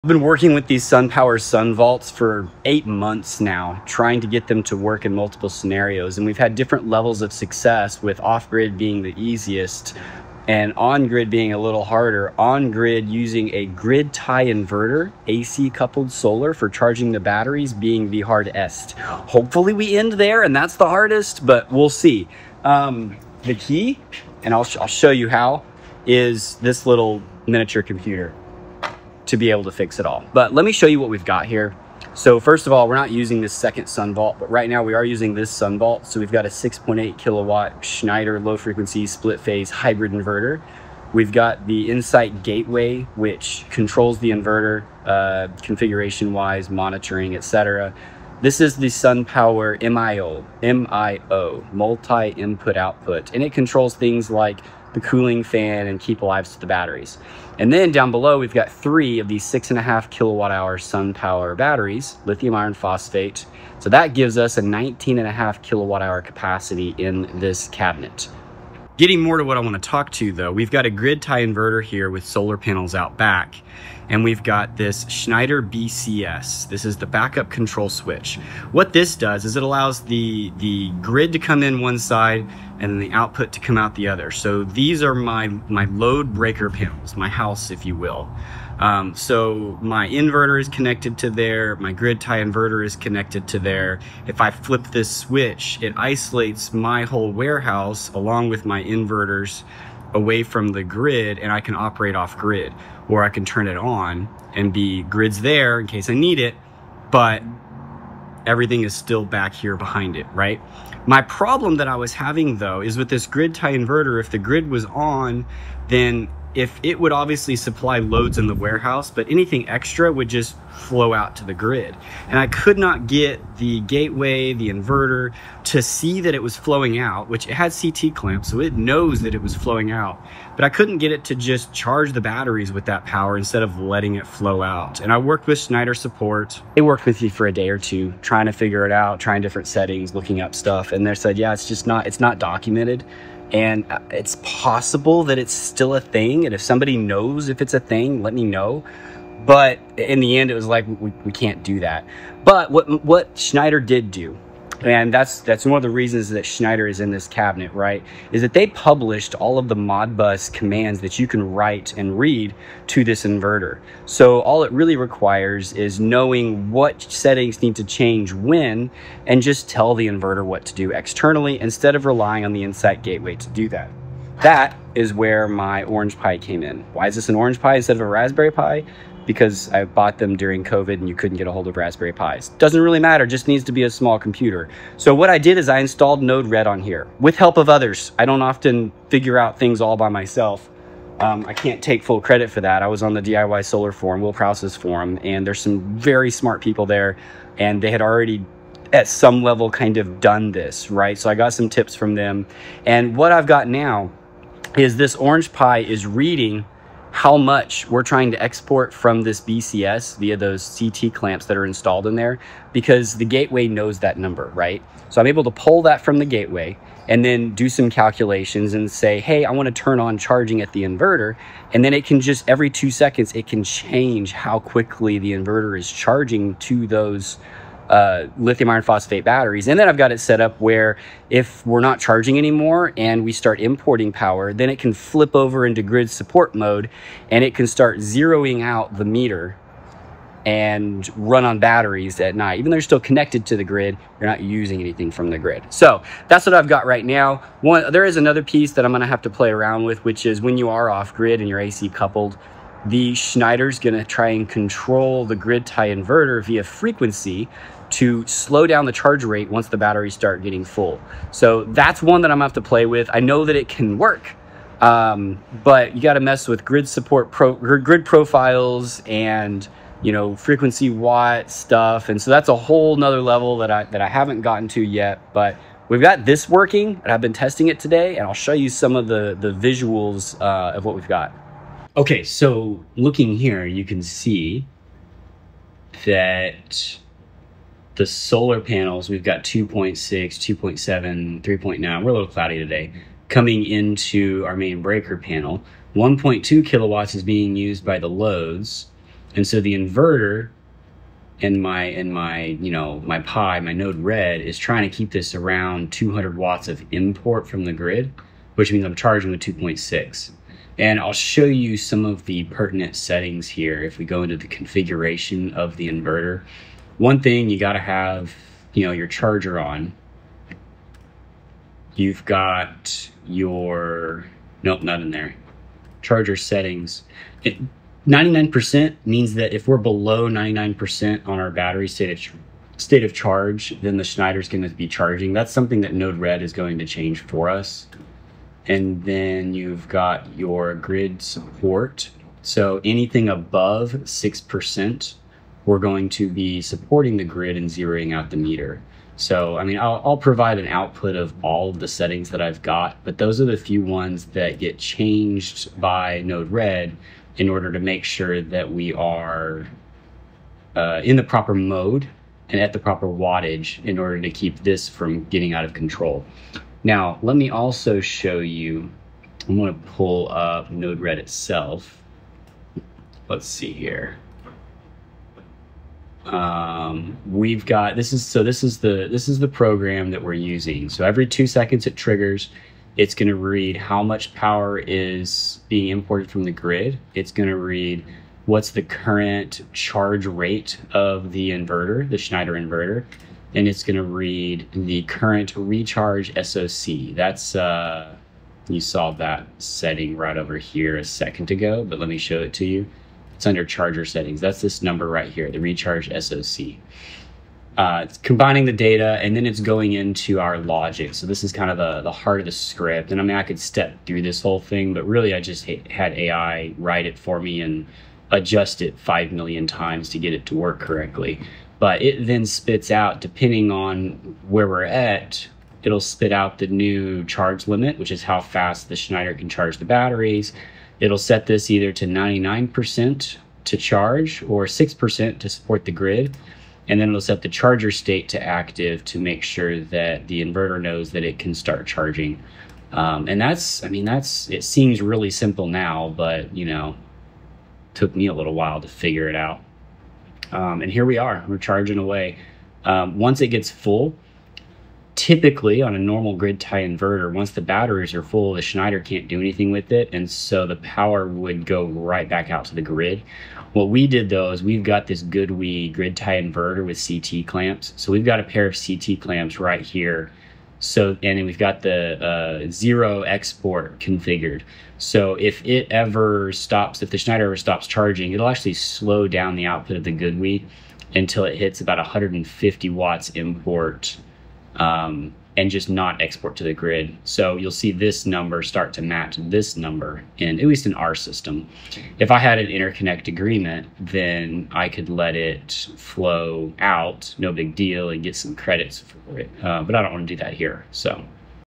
Adding Battery to Existing Solar
Beenebrothers.com/shop for many of your solar needs

More details about charging batteries with a SunVault/Schneider using a Grid Tie Inverter